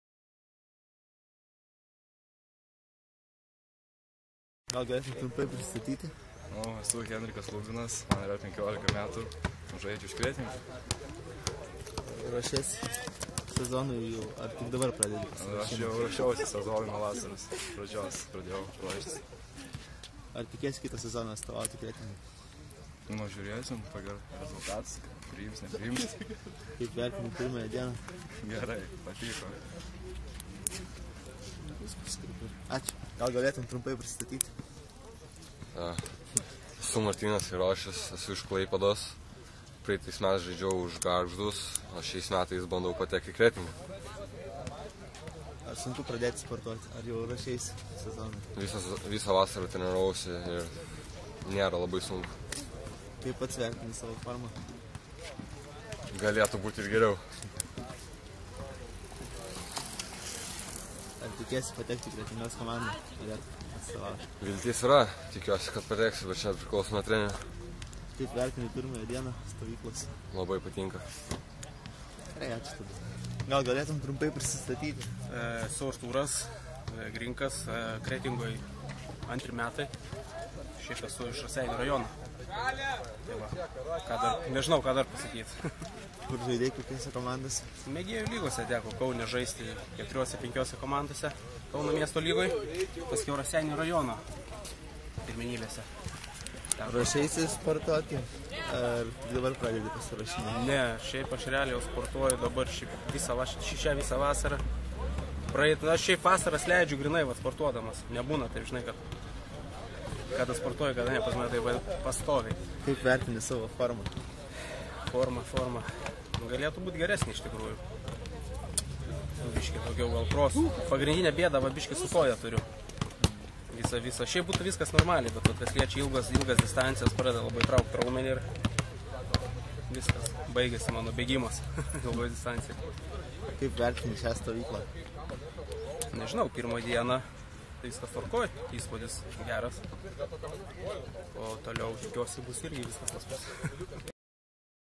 BC Kretingos stovyklos pirma diena. Kanditatų interviu
Po pirmos Kretingos komandos stovyklos/atrankos dienos paimta interviu iš trijų kanditatų: Henriko Lubino, Martyno Airošiaus ir Artūro Grinko. Kalbino Tomas Kubilius.